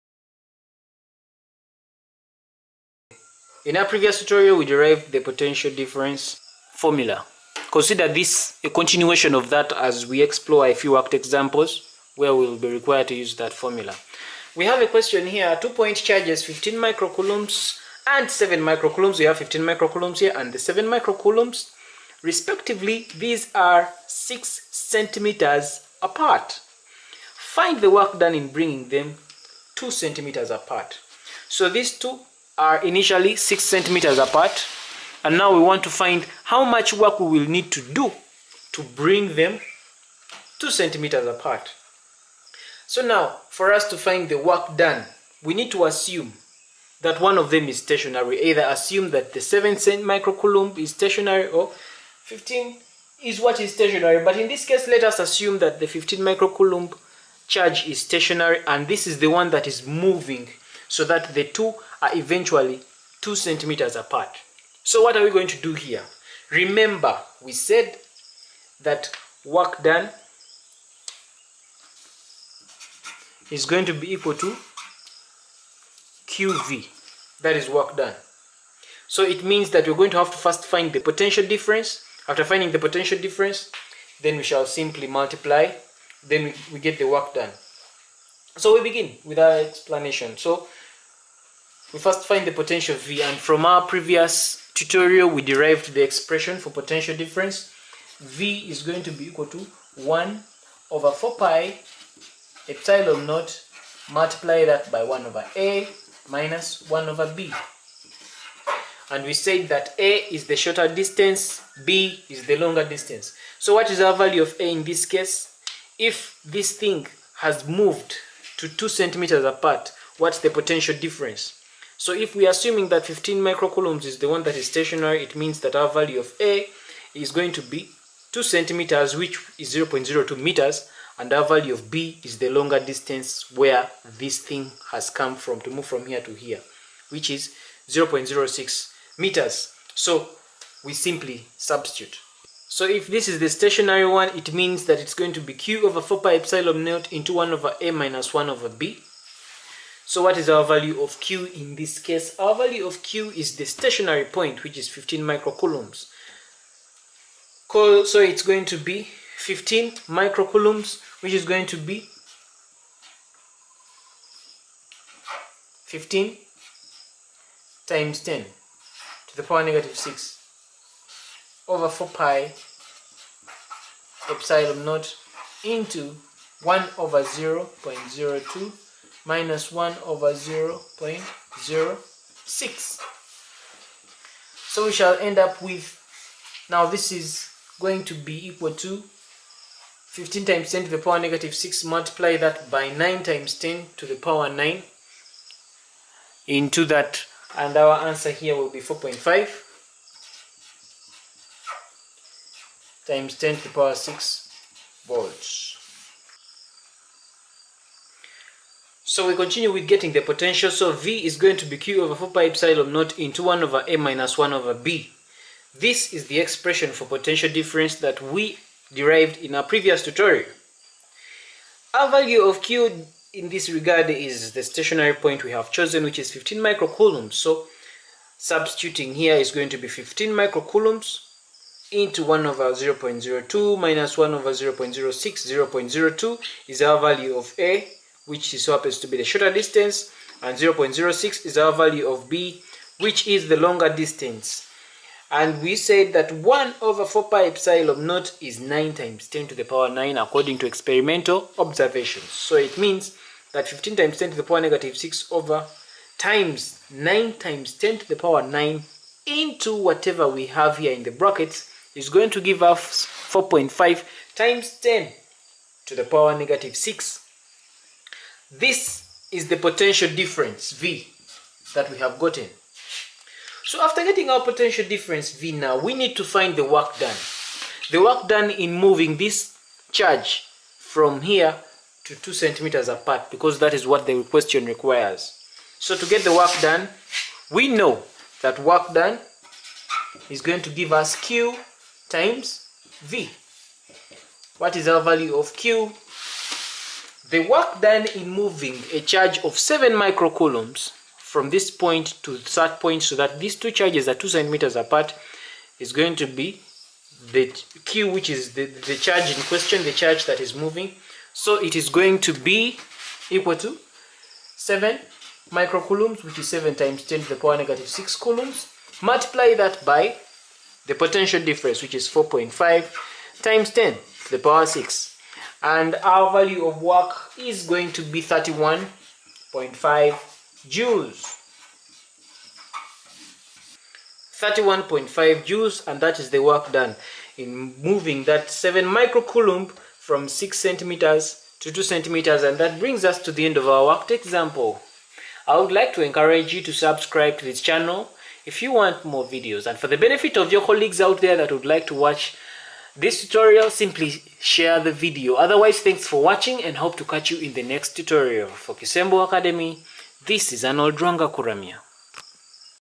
Electric potential difference formula | Potential difference between two point charges in a field
Potential difference between two point charges a and b in an electric field - kisembo Academy.

Thank you all who keep supporting this work via

Your generous contributions have facilitated the continued production of these videos. May God bless you more.

Potential difference between point charges - worked examples - Kisembo Academy
here we use the potential difference formula between two point charges. The volt by definition is the p.d between two points when the work done in moving one coulomb of charge from one point to another is one joule.